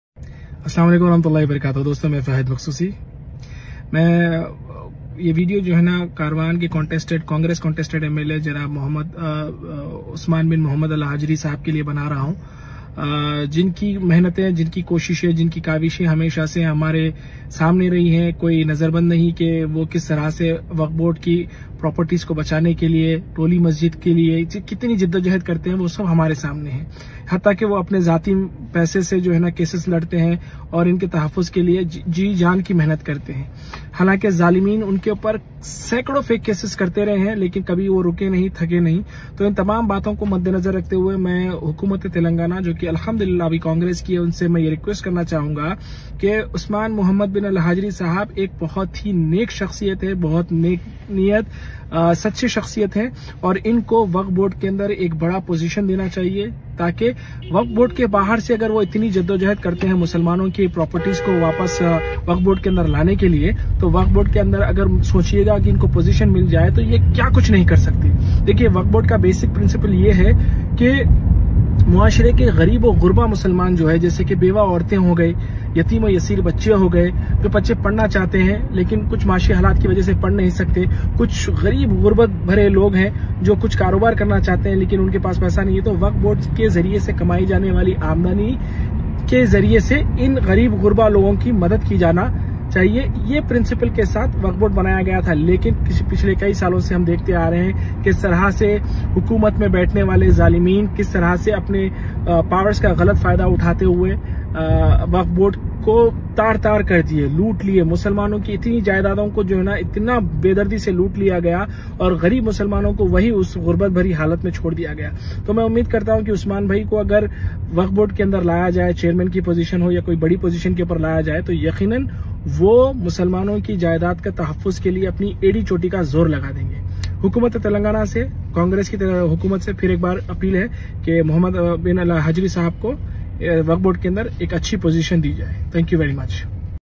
Osman Al Hajri Telangana Wakf Board Chairman keliye Ek Dum Saahi Candidate hai | Fahad Maqsusi
Osman Al Hajri Telangana Wakf Board Chairman keliye Ek Dum Saahi Candidate hai, Fahad Maqsusi ki Congress Sarkar se Appeal